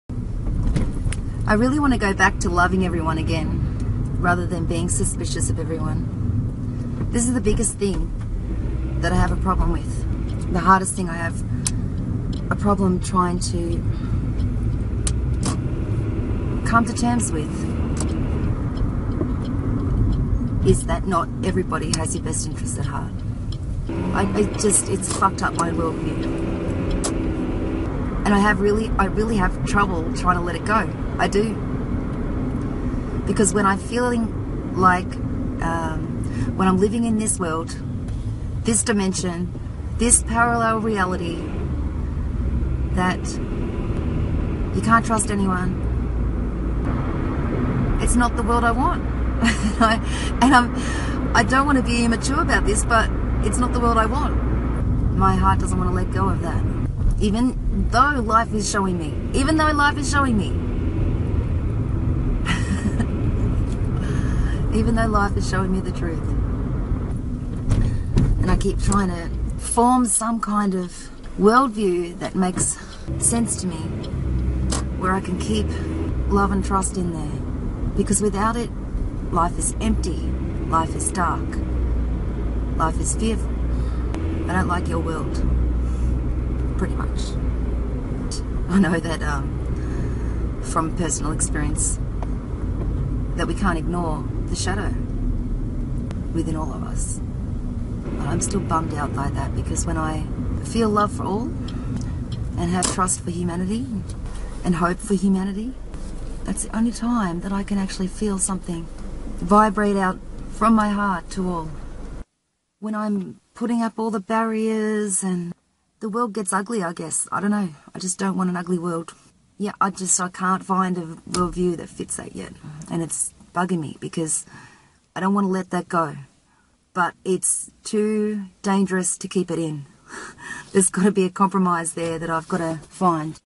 Can't Trust and it Bugs me - Don't want to live in hateful world
Struggling with this last quantum leap that I need to live joyfully without suspicion and distrust. Deleting the long-winded videos & re-uploading condensed re-caps. Originals are still on my blog. Nov 1st 2016